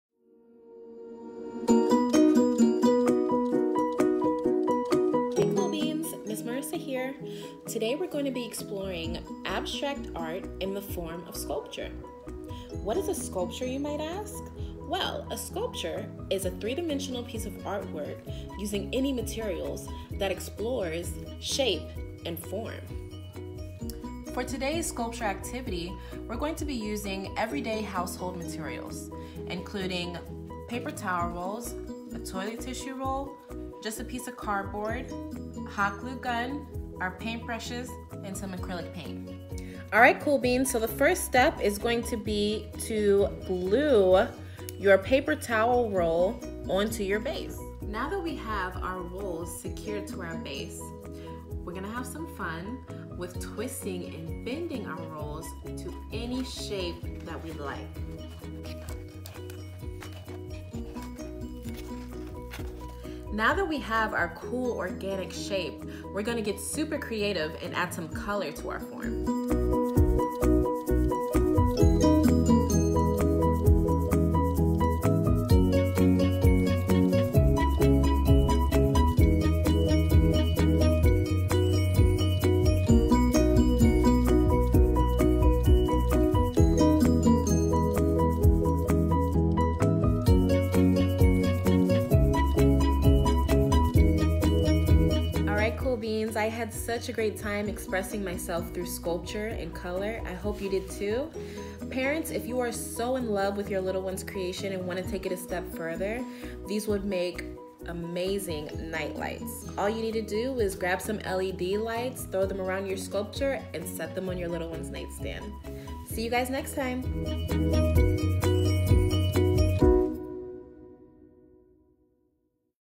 Sculpture Art Lesson
In today’s art lesson we explore what it means to create a sculpture art piece.